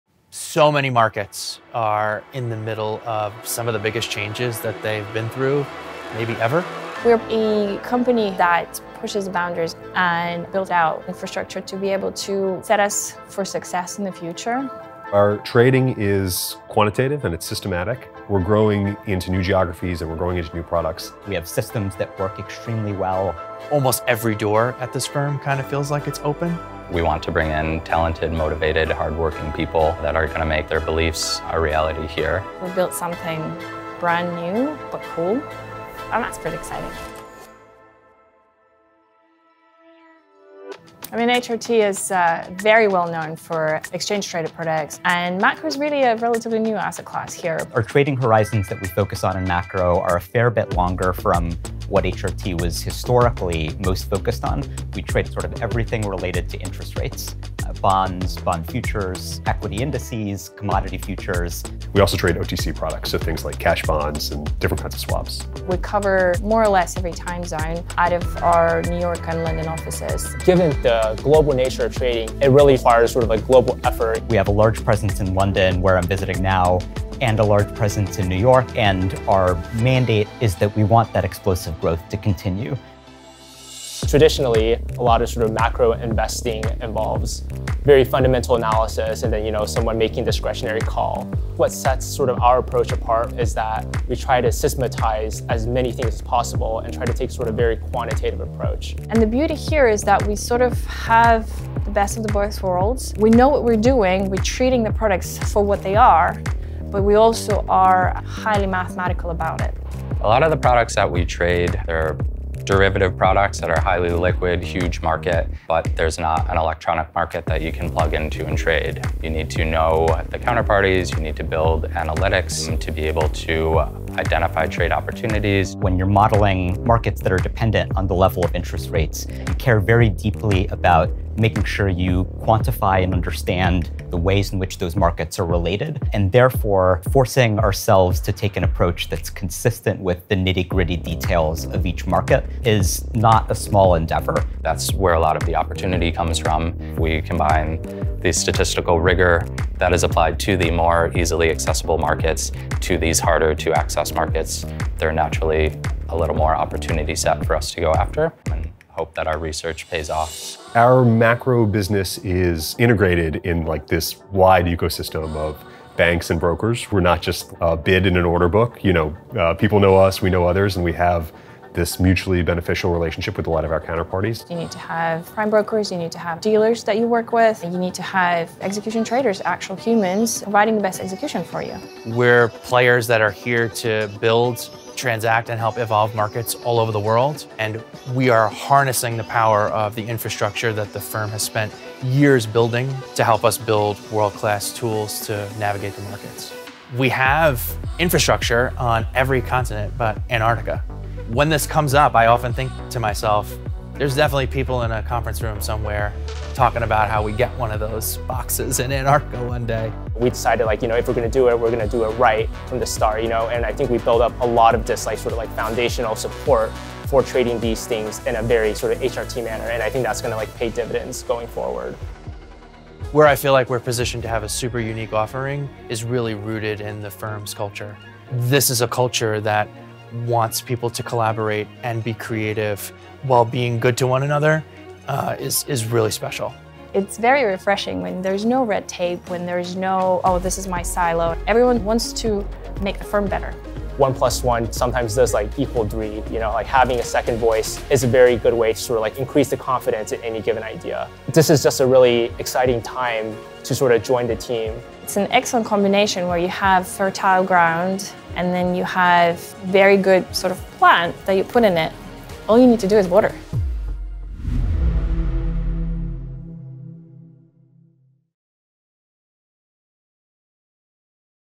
Macro Trading: HRT’s Approach to Global Markets
The Macro team is pushing the frontiers of what is possible at HRT, through new asset classes, global growth, and longer trading horizons.

They build data-driven strategies to trade financial instruments tied to entire countries and economies — with prices shaped by forces like interest rates, inflation, and global events. By applying advanced mathematical modeling, a deep understanding of market dynamics, and the full power of HRT's industry-leading infrastructure, Macro develops cutting-edge trading strategies in new markets spanning almost every continent.